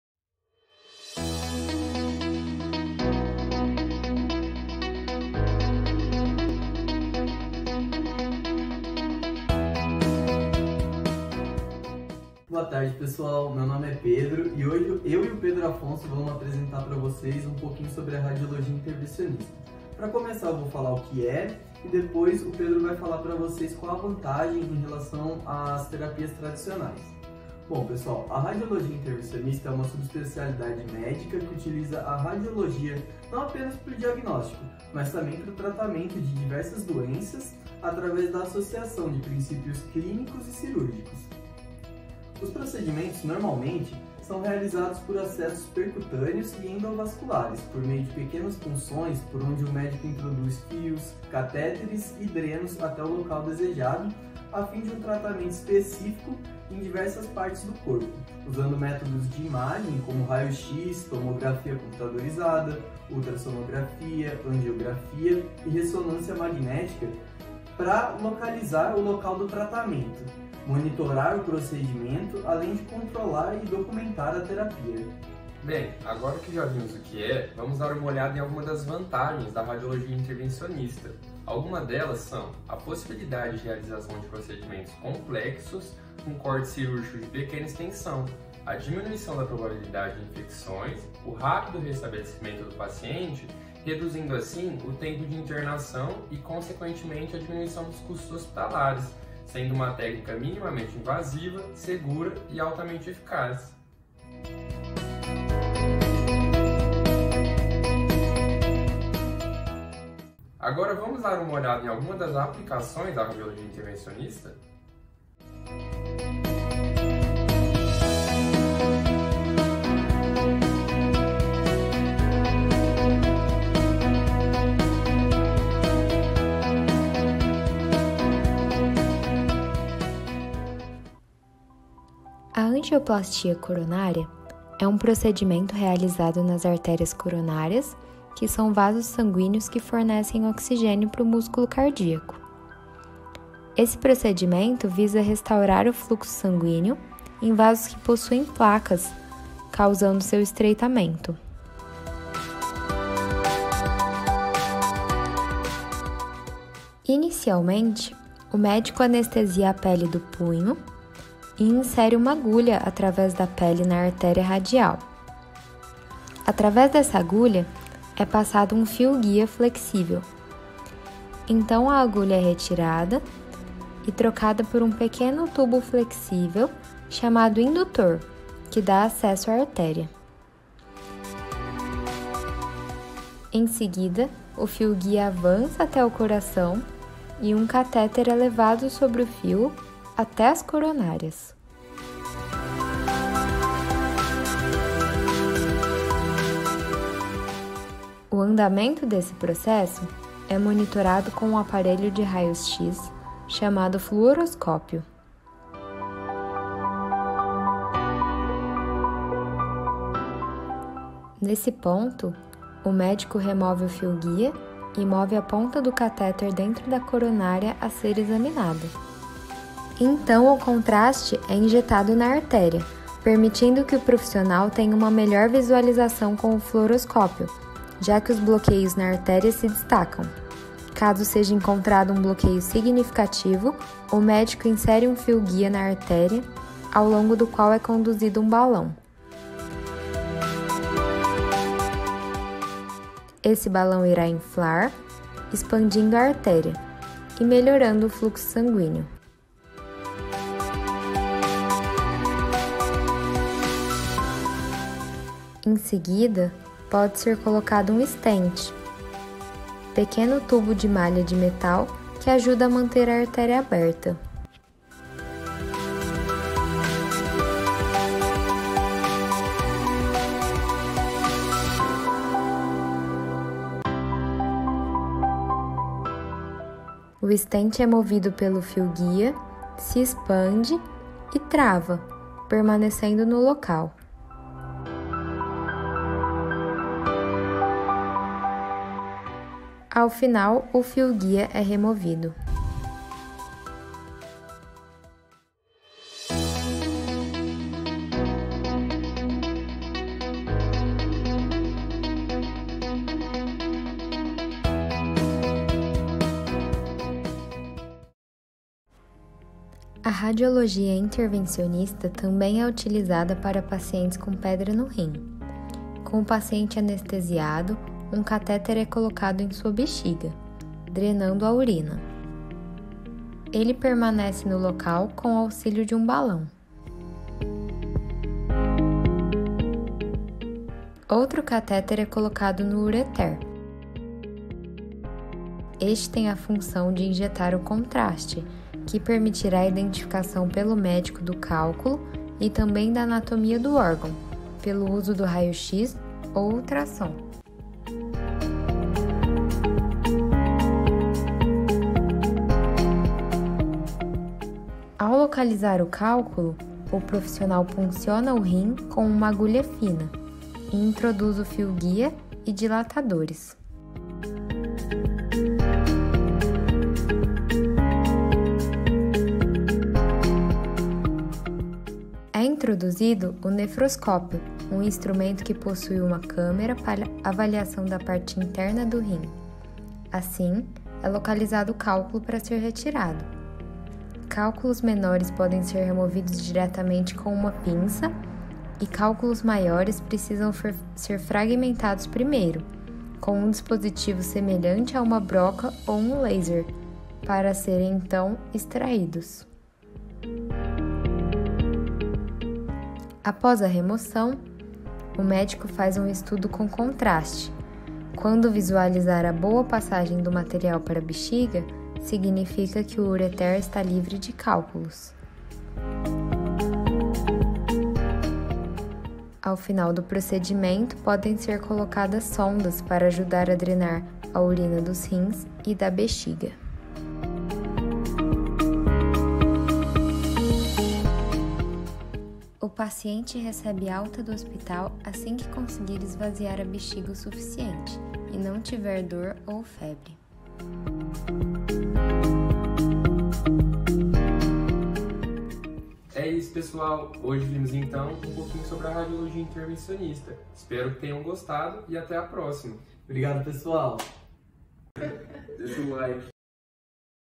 Radiologia Intervencionista
Video produzido pelos excelentes alunos da medicina uftm, explicando sobre essa subespecialidade da radiologia que cada dia é mais relevante.
Grupo:

Arthur: levantamento teórico da apresentação; seleção do texto a ser apresentado; auxílio
às demais partes.
Bárbara: levantamento teórico dos procedimentos a serem abordados; seleção dos vídeos;
edição dos vídeos e da apresentação; montagem da apresentação; auxílio às demais
partes.
Fabiane: levantamento teórico dos procedimentos a serem abordados; seleção dos vídeos;
partes.
Júlia: levantamento teórico dos procedimentos a serem abordados; narração dos vídeos;
filmagem; montagem da apresentação; auxílio à edição; auxílio às demais partes.
Leoncio: levantamento teórico do material a ser utilizado na apresentação, leitura e estudo
do material, gravação e apresentação do material, auxílio às demais partes.
Pedro Afonso: levantamento teórico da apresentação; narração e gravação do vídeo;
auxílio à montagem da apresentação; auxílio às demais partes.
Pedro Henrique: levantamento teórico da apresentação; narração e gravação do vídeo.